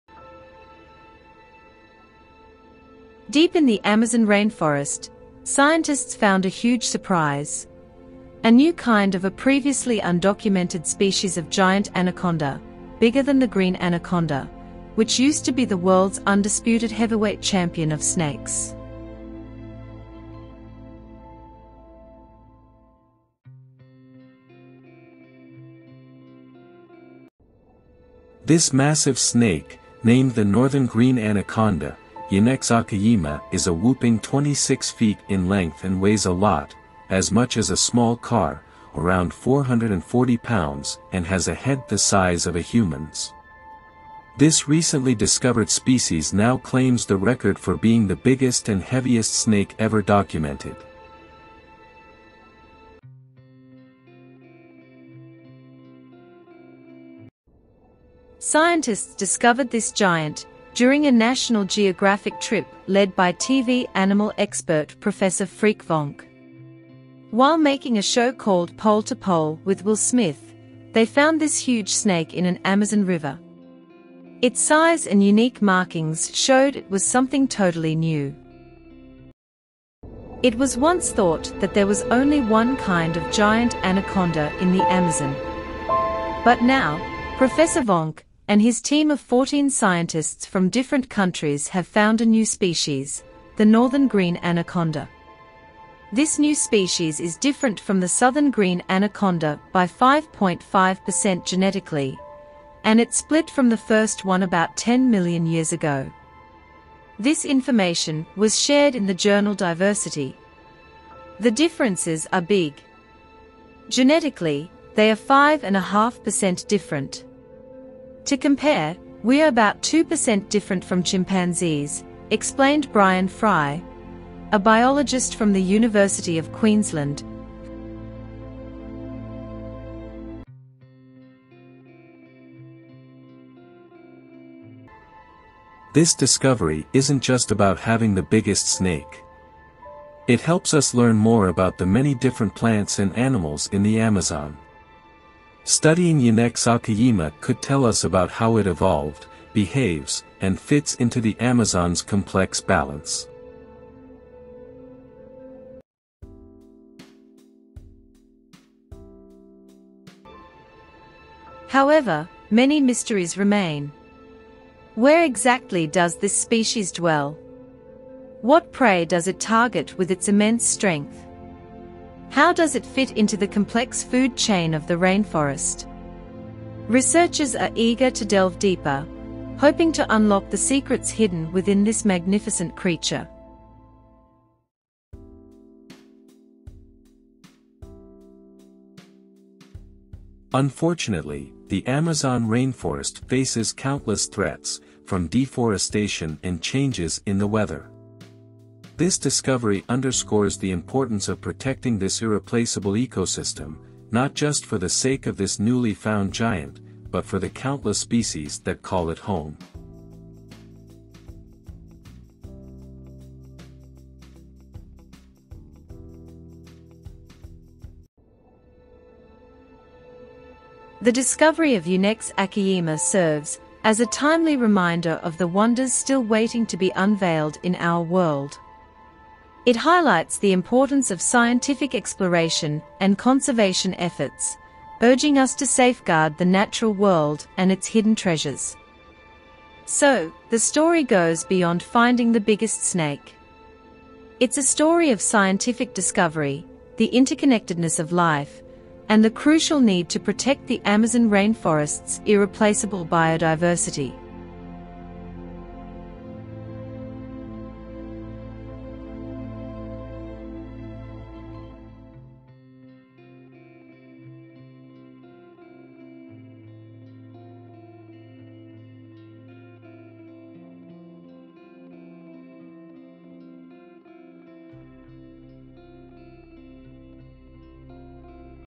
World's Largest and Heaviest Snake Species Discovered in Amazon Rainforest.
The Science Connect TV - Research News - World's largest and heaviest snake species discovered in amazon rainforest.
Deep in the Amazon rainforest, scientists found a huge surprise: a new kind of a previously undocumented species of giant anaconda, bigger than the green anaconda, which used to be the world's undisputed heavyweight champion of snakes.
This massive snake, named  the northern green anaconda, Eunectes akayima  is a whooping 26 feet in length and  weighs a lot – as much as a small car, around  440 pounds and has a head the size of a human's.  This recently discovered species now claims the record for being the biggest and heaviest snake ever documented.
Scientists discovered this giant during a National Geographic trip led by TV animal expert Professor Freek Vonk. While making a show called "Pole to Pole" with Will Smith, they found this huge snake in an Amazon river. Its size and unique markings showed it was something totally new.